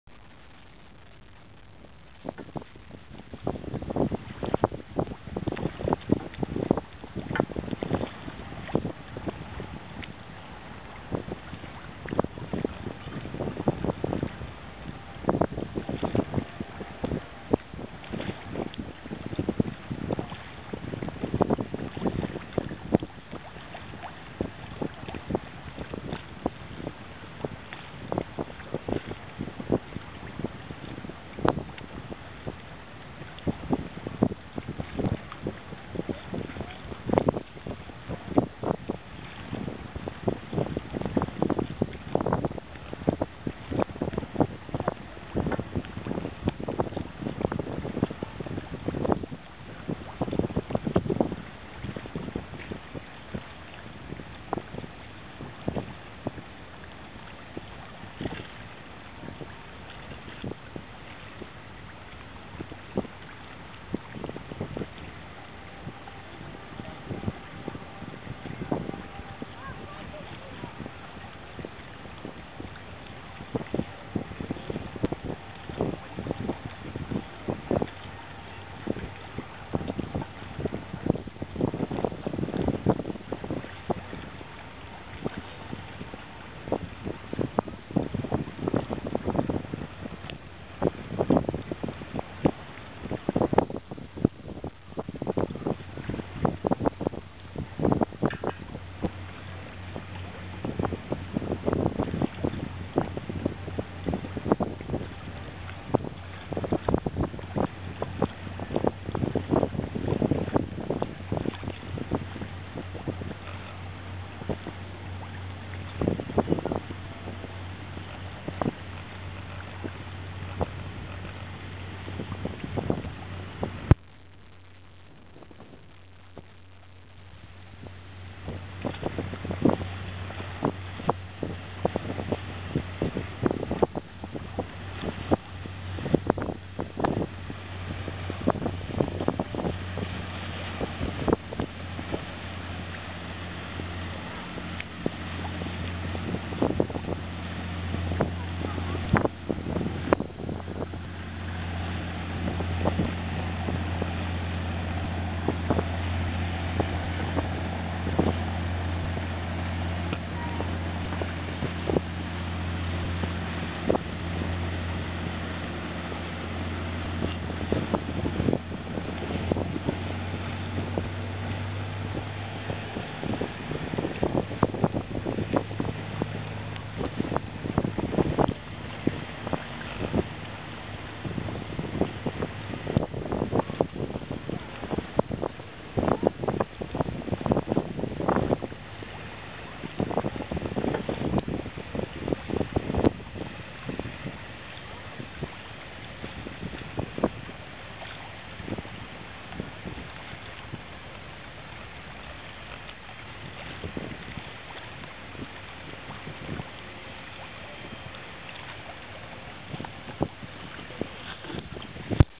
入港・その１
2009年5月31日、和歌山港で実施された、護衛艦ちょうかいの体験航海の様子です。